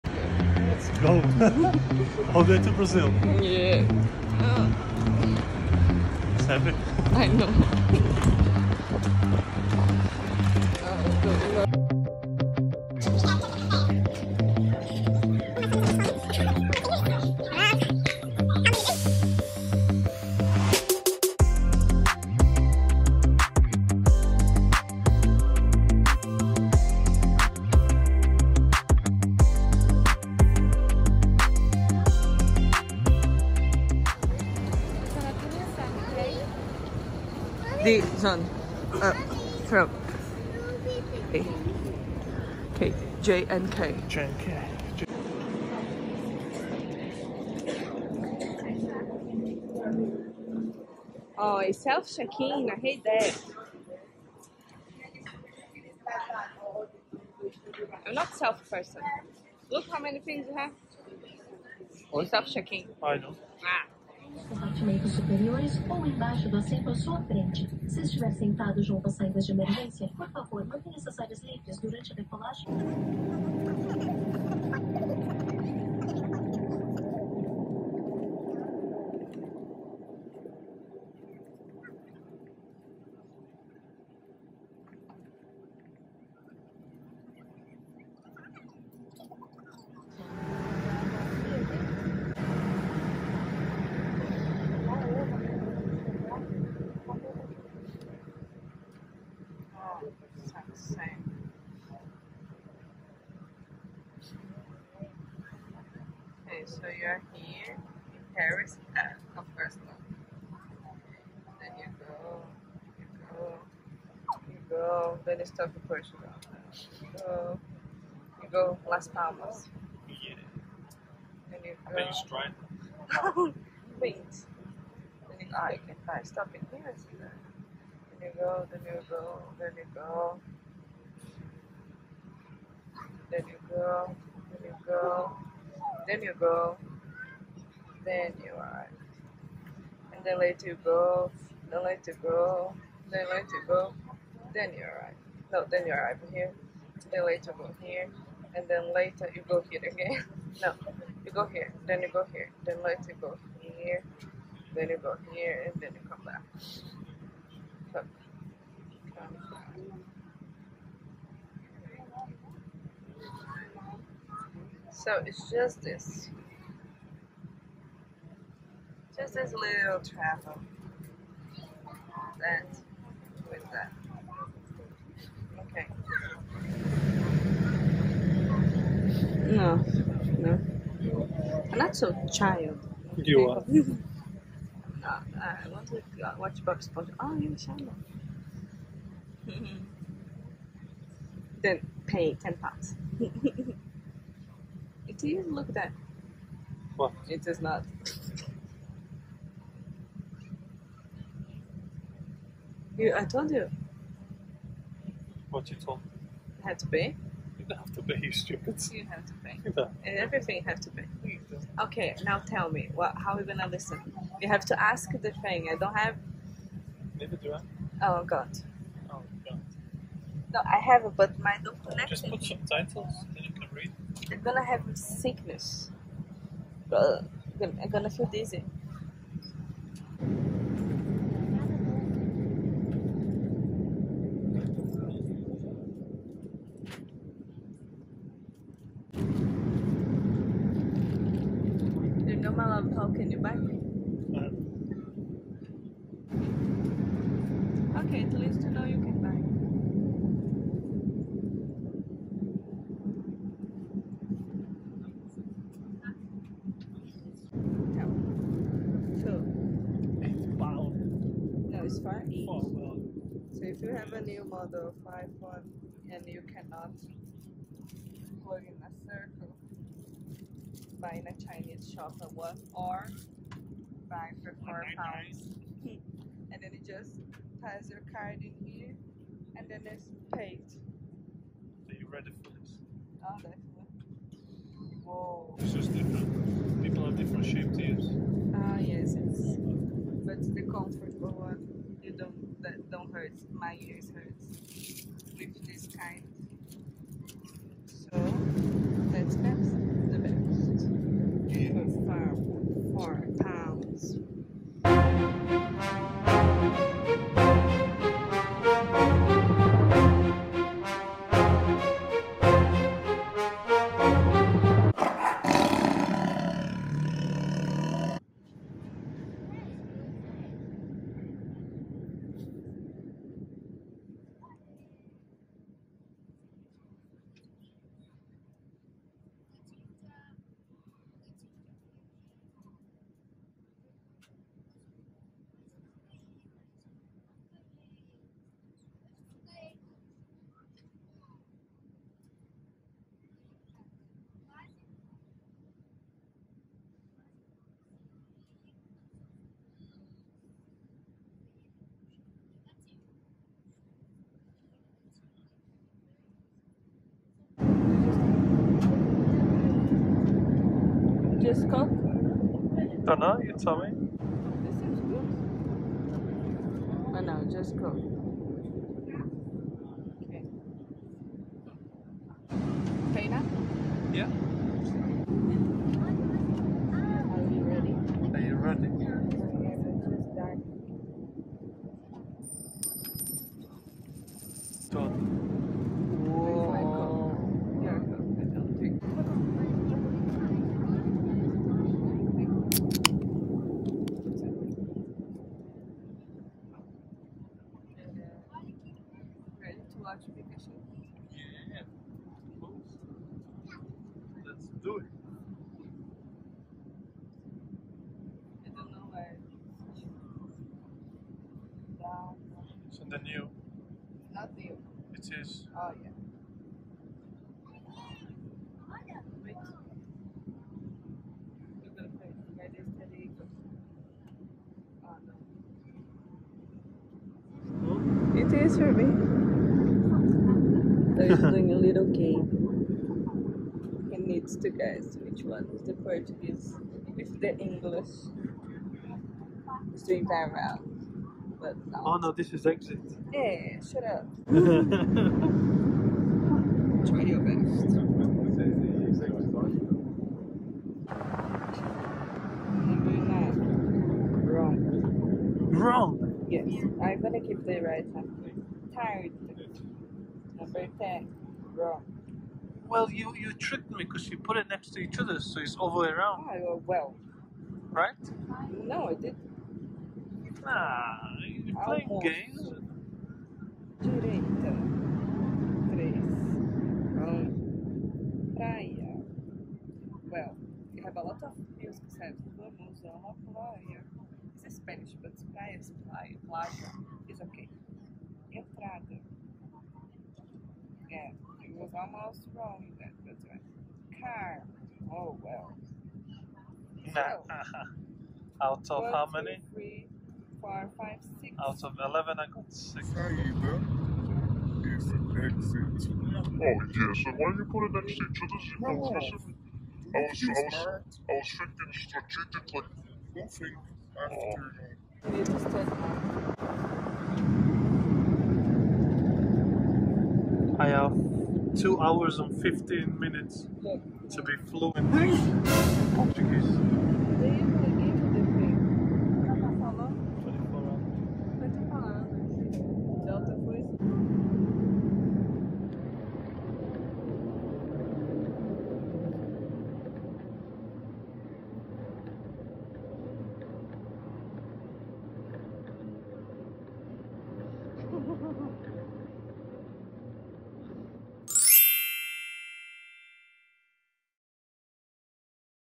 TRAVEL TO BRAZIL- FLIGHT VLOG!
Life Before Covid! Que saudades de viajar e de não precisar usar máscara, principalmente de visitar a família. Viajamos pela Norwegian num voo direto! Esse vídeo e nos vídeos futuros serão meus tbt da viagem de 2019! Lembrar é viver né noon!

 Se cuidem! :)